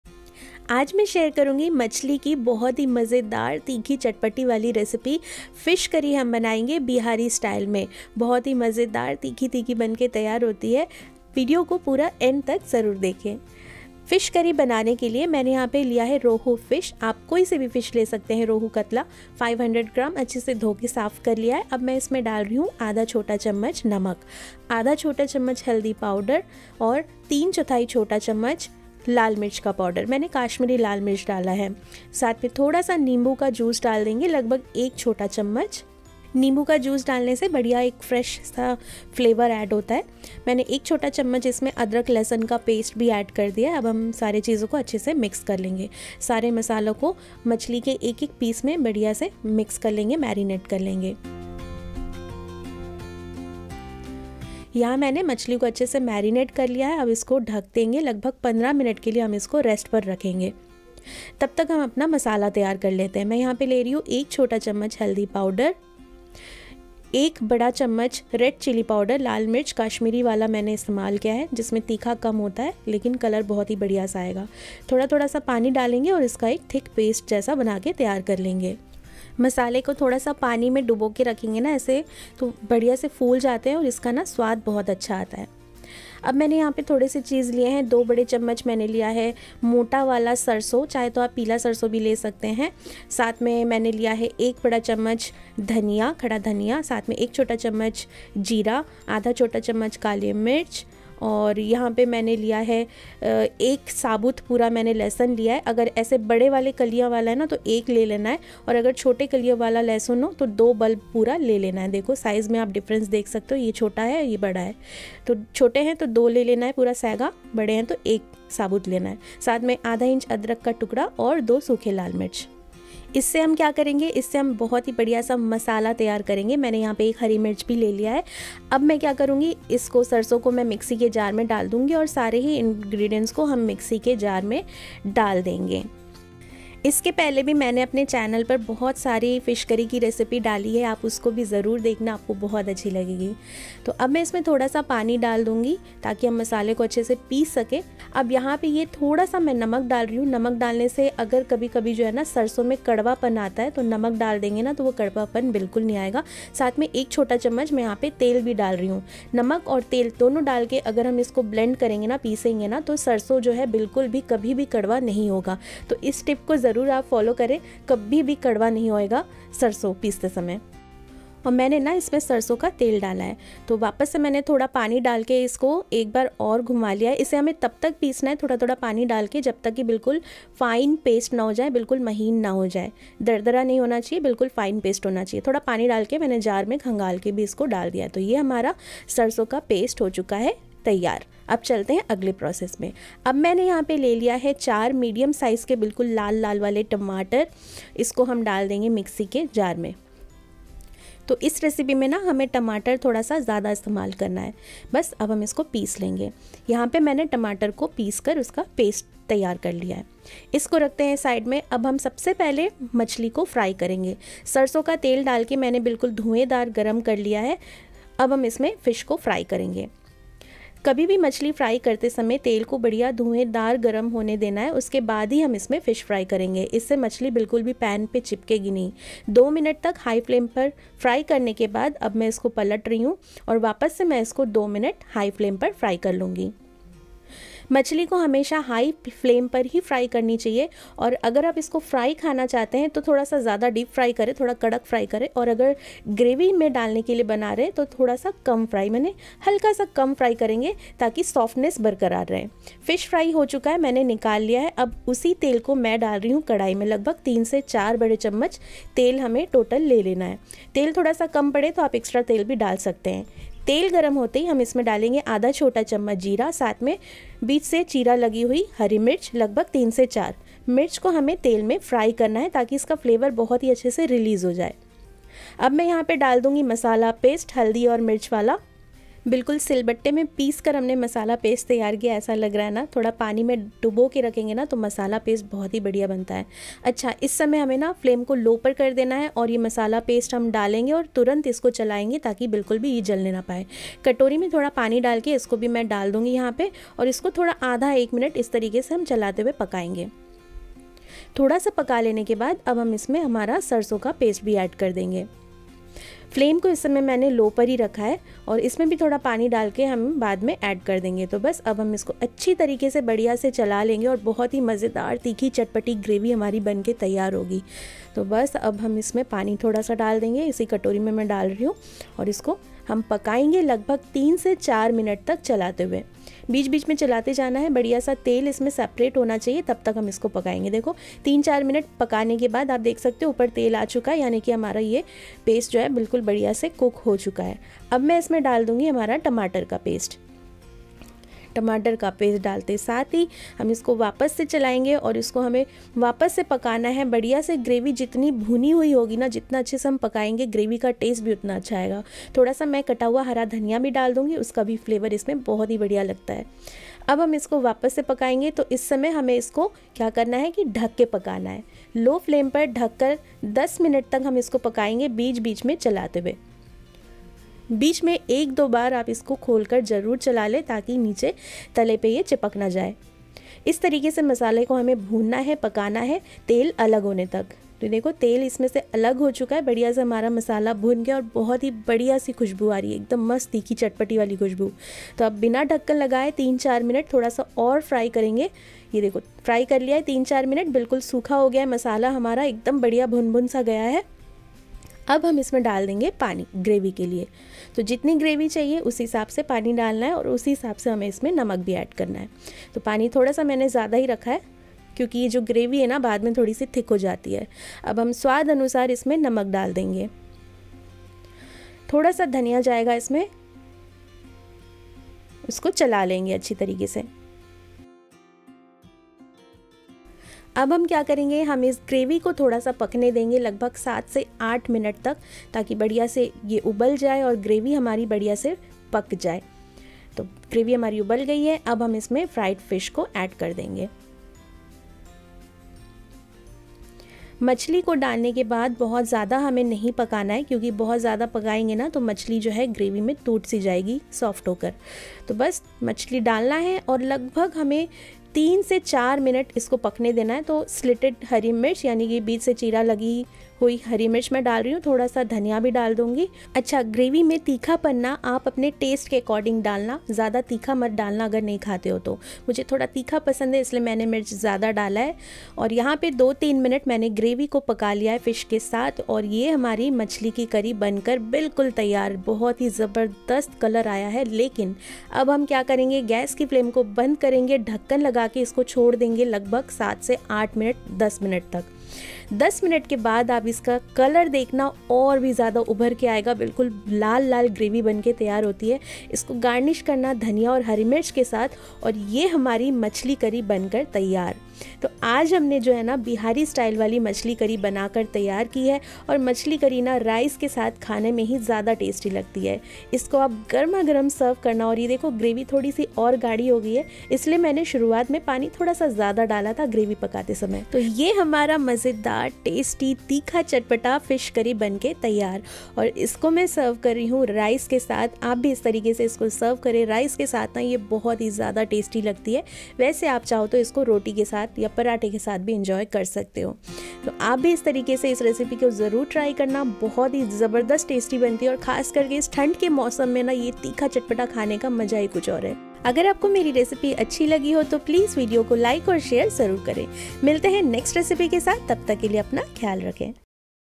Bihari Fish Curry | बिहारी स्टाईल में सबसे स्वादिष्ट मछली करी | Rohu Fish Curry
Ingredients:
Rohu fish 500gm
Turmeric 2 tsp
Red chilli powder 1tsp + 1 tbsp
Salt
Mustard oil as required
Tomato - 3
Coriander leaves